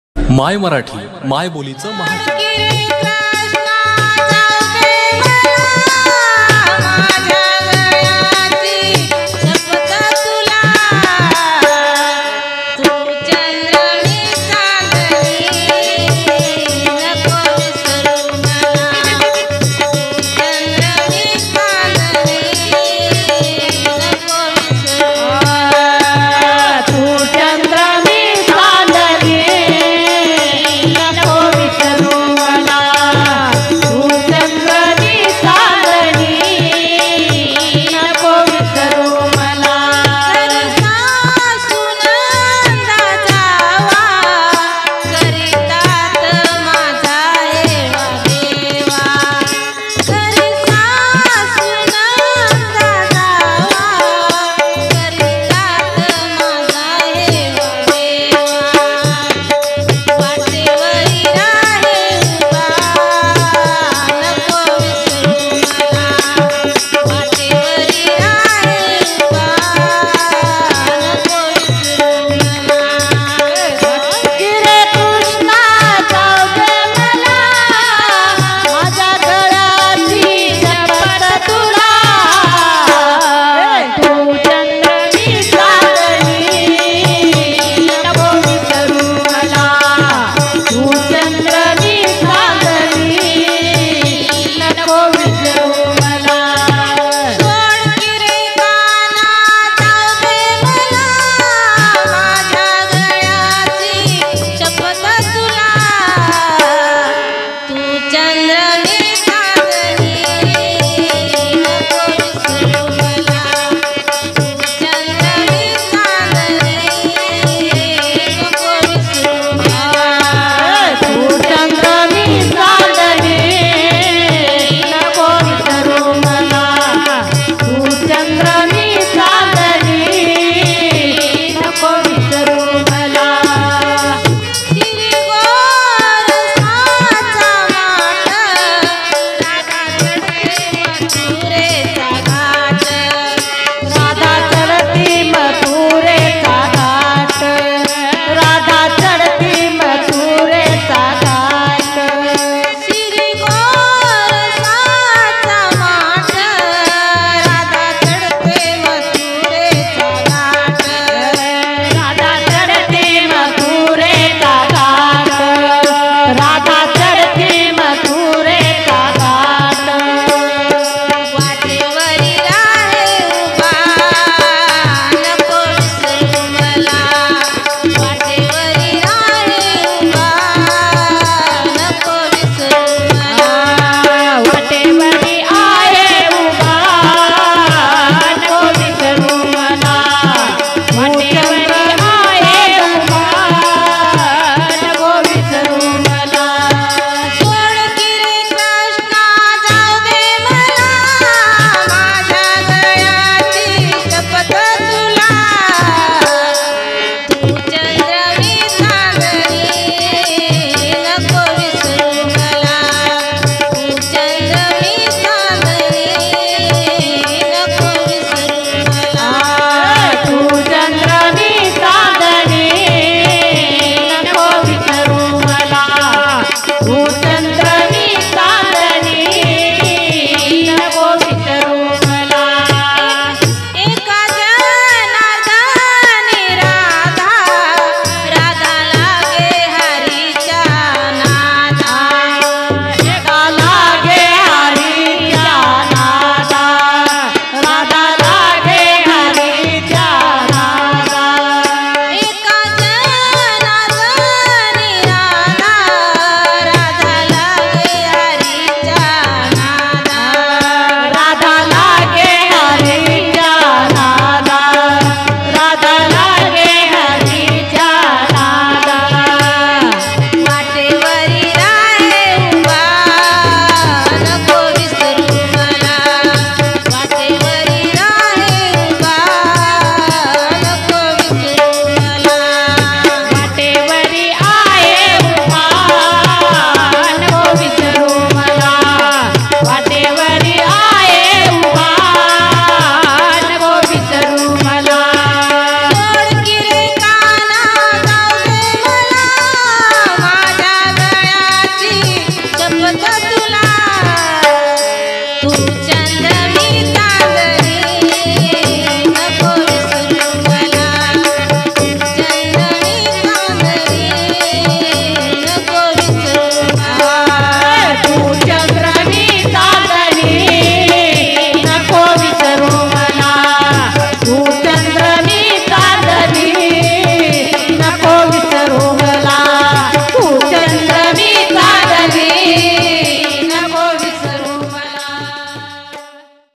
gavlan,सीमा विघ्वे,seema vighwe,folk song, devotional song,bhajan,bharud,भजन,भक्तिगीत,भावगीत,गवळणी,
सीमा विघ्वे .. लातूरच्या अहमदपूर तालुक्यातील हडोळतीच्या रहिवासी असलेल्या सीमा अभंग,गवळण, भावगीत, भक्तीगीत गातात.
सीमा यांनी सादर केलेली ही सुपरहिट गवळण खास आपल्यासाठी.

'माय मराठी '.
मायबोलीचं महाचॅनल. !!

mai Marathi...
the biggest channel of the mothertoung.
-rajesaheb kadam